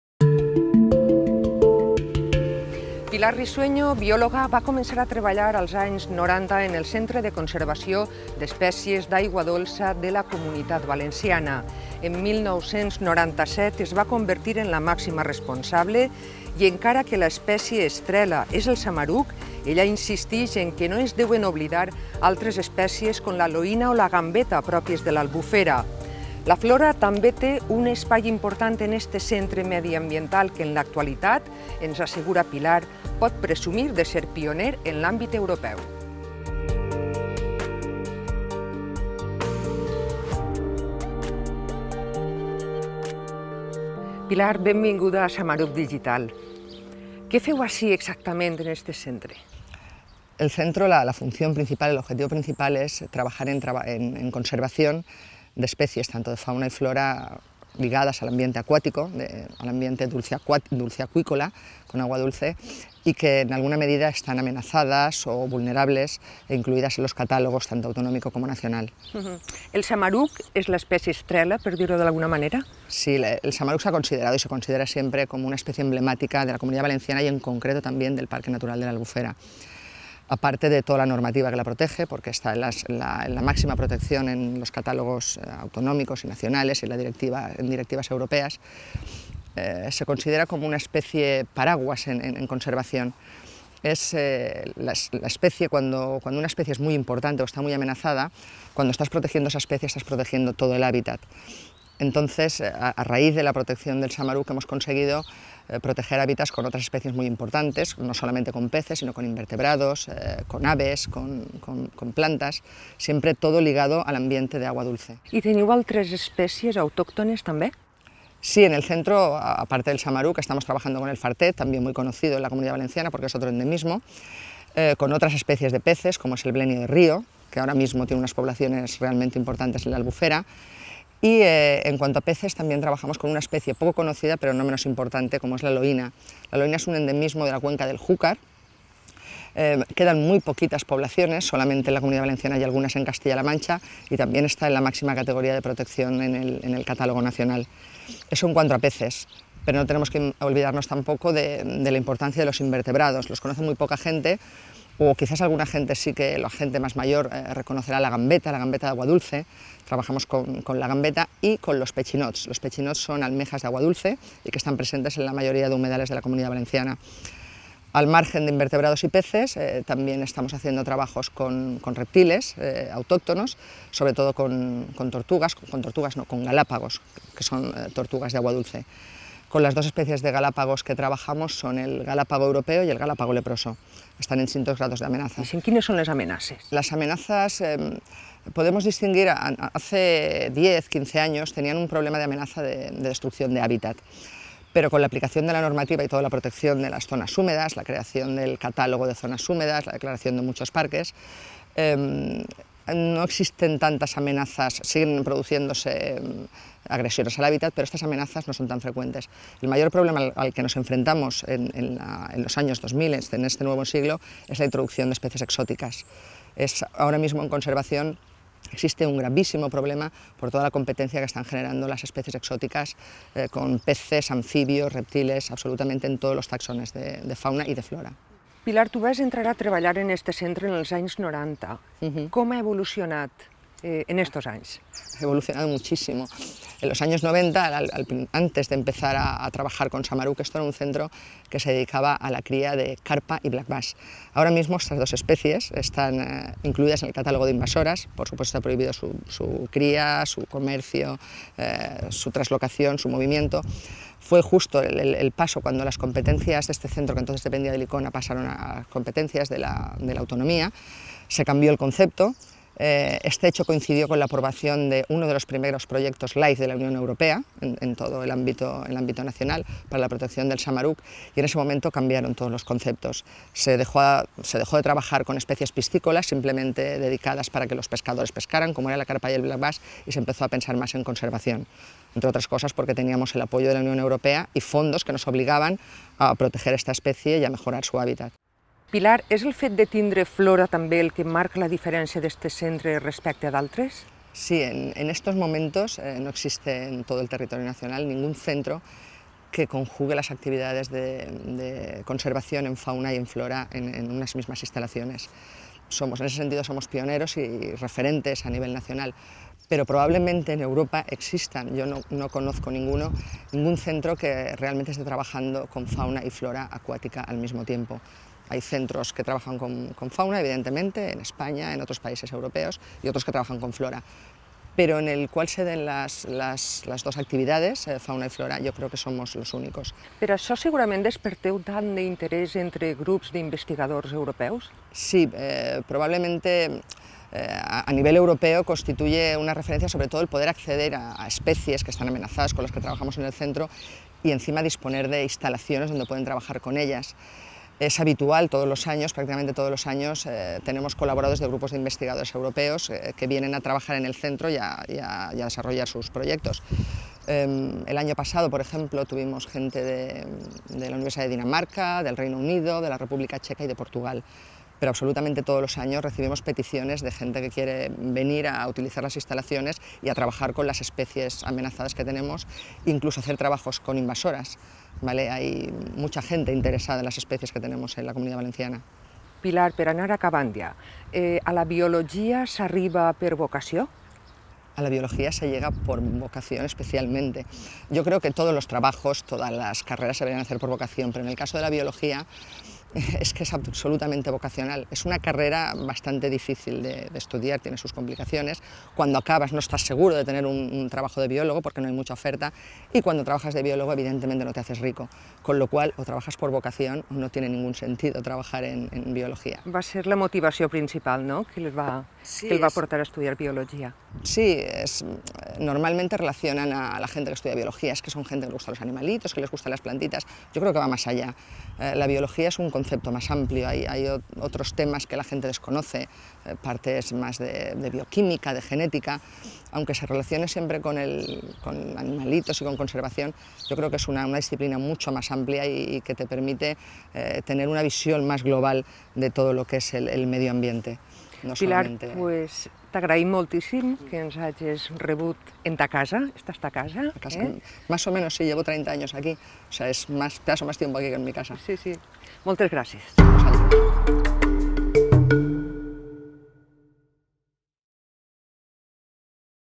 Pilar Risueño: la conservació de la vida d’aigua dolça
Responsable del Centre d'Espècies d'Aigua Dolça de la Comunitat Valenciana, aquesta prestigiosa biòloga afirma que la seua professió s'ha d'exercir des de la més absoluta vocació. El projecte conservacionista que dirigeix està considerat pioner a l'àmbit europeu, i és tot un referent pel que fa a la conservació d'espècies de flora i fauna i l'estudi d'estratègies per a la protecció del seu hàbitat.